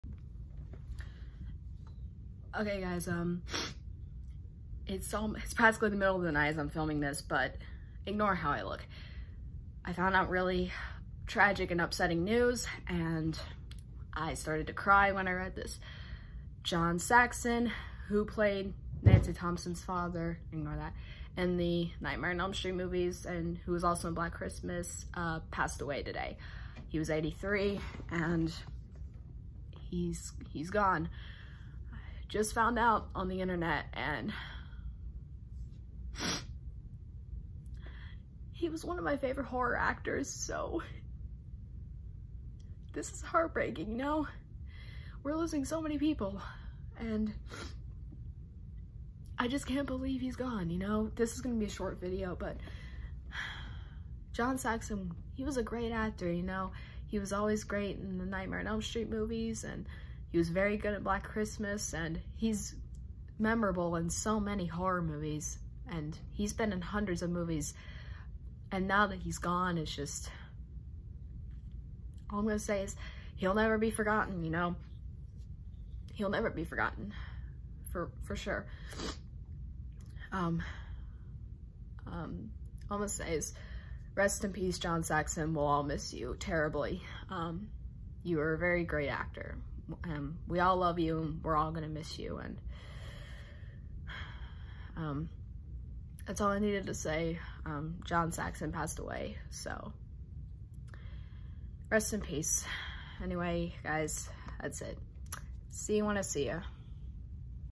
Rest In Peace John Saxon
We lost another horror legend today! May he Rest In Peace!